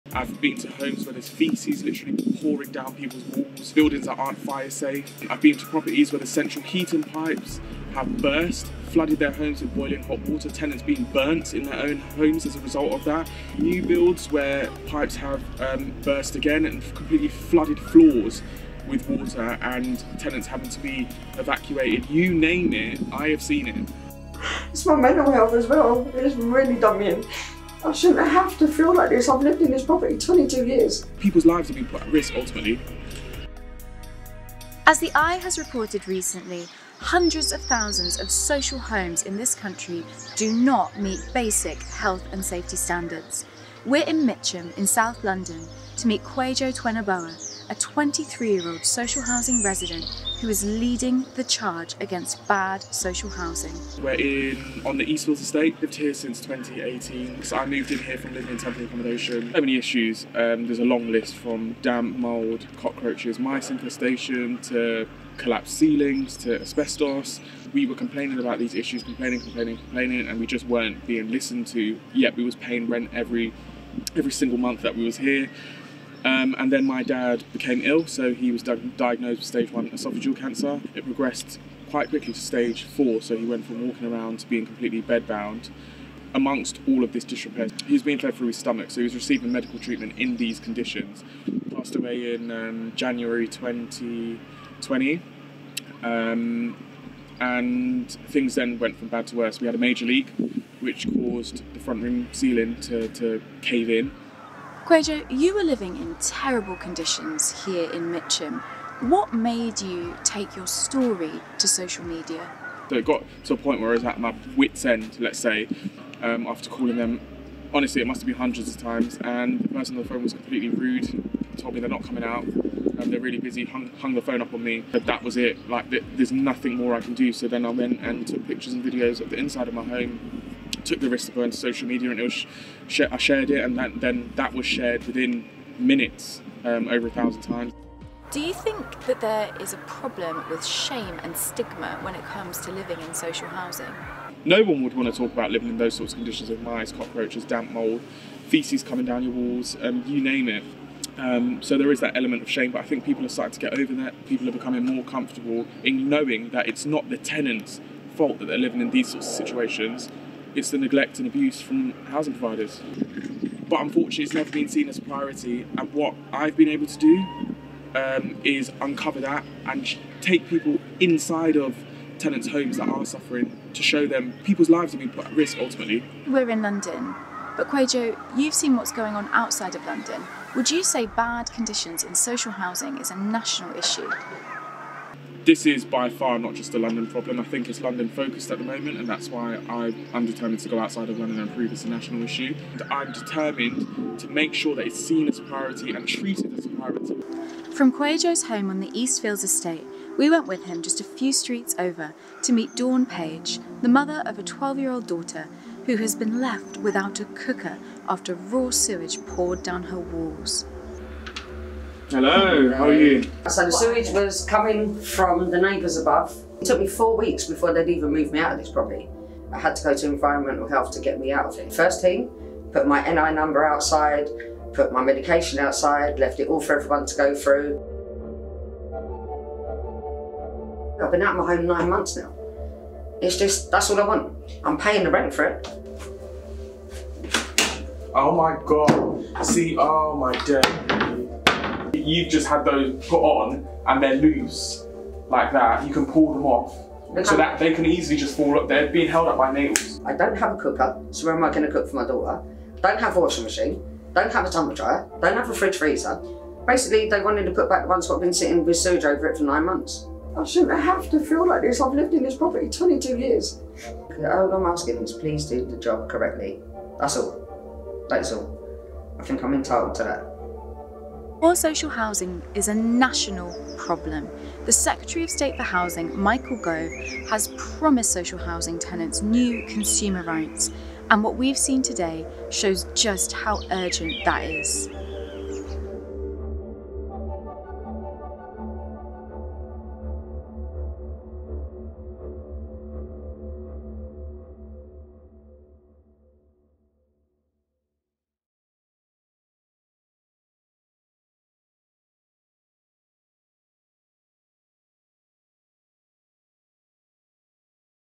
Meet Kwajo Tweneboa: the 23-year-old taking on the UK's bad social housing
🏠 i's Housing Correspondent Vicky Spratt meets Kwajo Tweneboa, the 23-year-old activist lending his voice to tenants living in poor social housing.

📱 On Twitter, Instagram and TikTok Kwajo shares videos of atrocious slum-like housing conditions, holding those responsible to account. The images are impossible to unsee, raw sewage running down walls, black mould so thick it looks as though you could spoon it off surfaces, rats, mice, cockroaches and toilets overflowing.

🏢 Since Michael Gove took on the role of Secretary of State for Levelling Up, Housing and Communities hopes were raised among housing activists that renters’ rights would finally see a much needed reform. With Gove now sacked from his post in the final weeks of Boris Johnson’s premiership the future now looks uncertain for Britain’s most vulnerable tenants.

📰Check out more of Vicky Spratt’s work at iNews, including exclusive access to an excerpt from her new book ‘Tenants’, an astute look at Britain’s housing crisis.